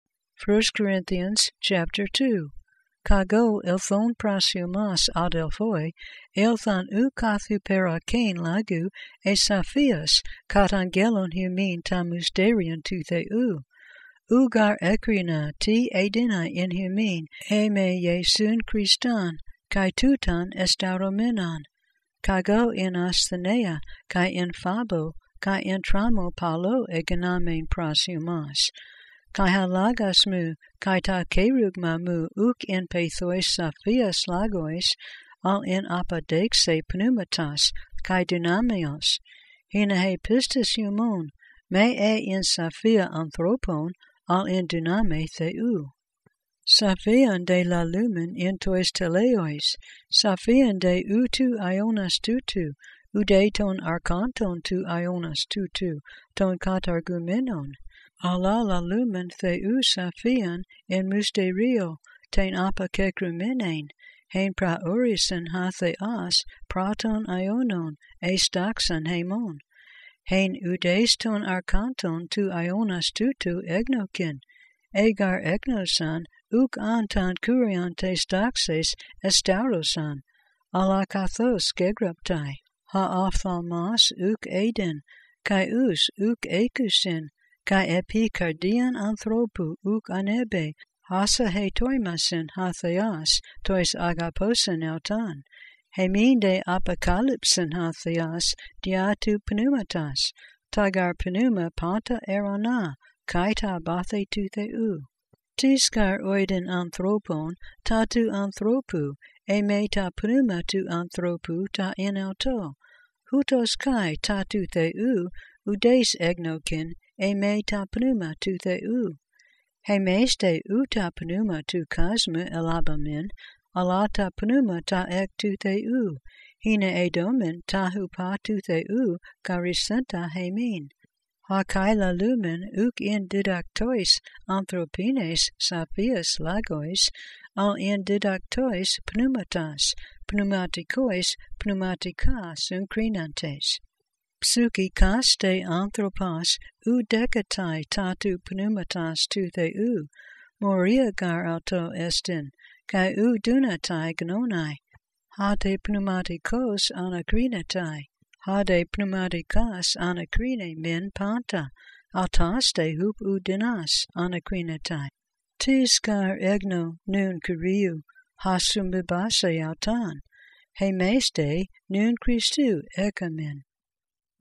1 Corinthians 2 - Interlinear Greek New Testament Bible
1 Corinthians 2 - Interlinear Greek New Testament Bible - Erasmian Pronunciation

Preached Christ crucified, not wisdom. Wisdom to mature by Spirit. Spiritual things folly to the natural.